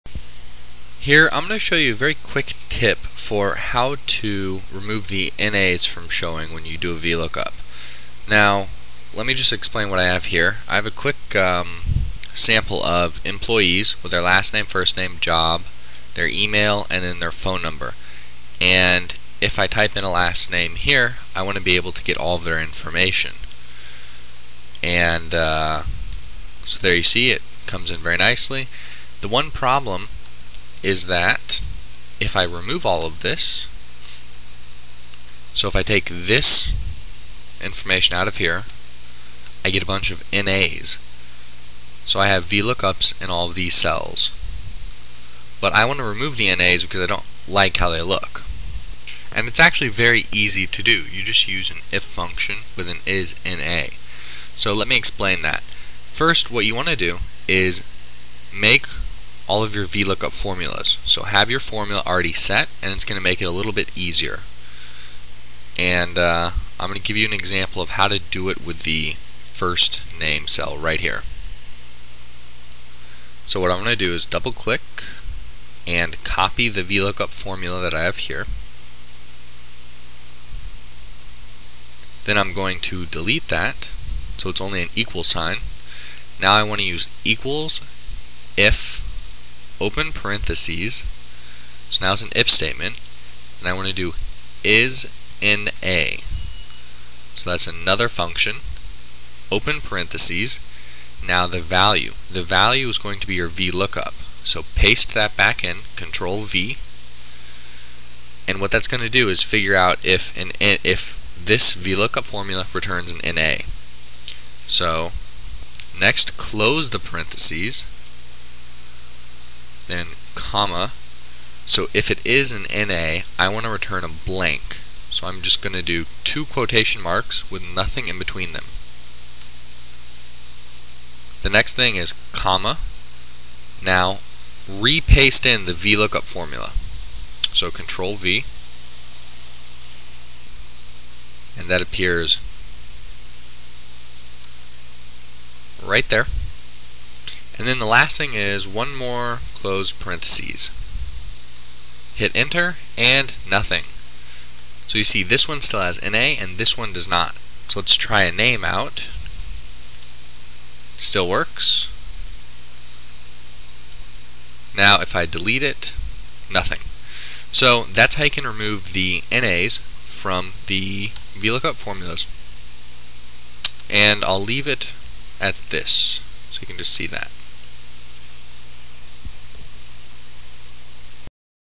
Remove the N/A Error from VLOOKUP in Excel
How to remove the N/A value from cells in excel which contain vlookup formulas. To do so, you need to use and IF statement and an ISNA function. This tutorial teaches you how to do that.

For more or to follow along with the spreadsheet used here, go to the website below: